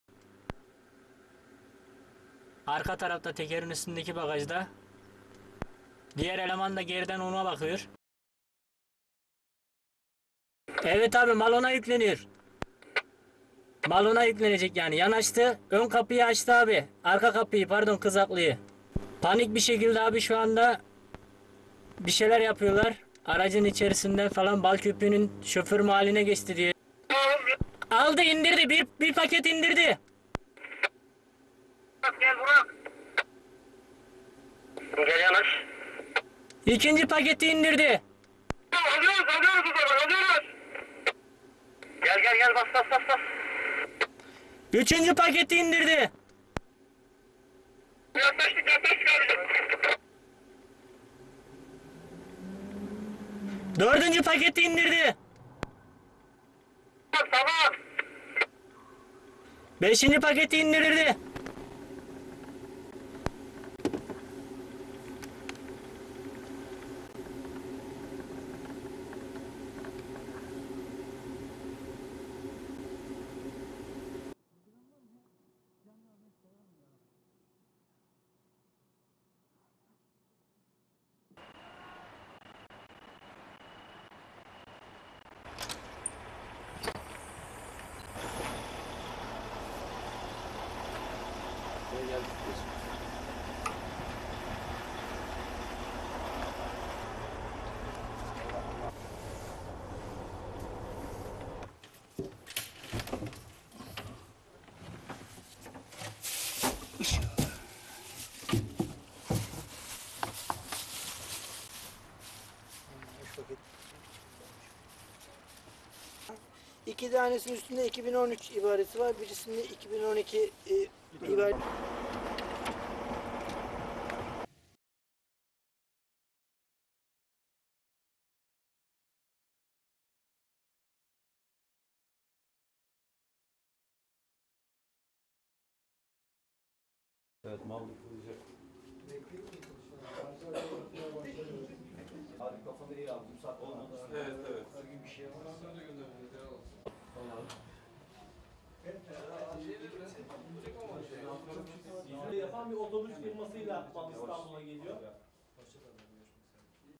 Polizei in Istanbul lässt Drogenschmuggler hochgehen und beschlagnahmt 307 Kilo Heroin
Die türkische Polizei hat einen Drogenschmugglerring hochgehen lassen. Auf dem Polizeivideo sieht man wie Unbekannte von einem Mini-LKW Drogen in einen Transporter verladen. In dem Moment schnappt die Polizei auch zu und stellt die Ware sicher: 307 kg Heroin. Die Drogen waren auf dem Weg in die Niederlande.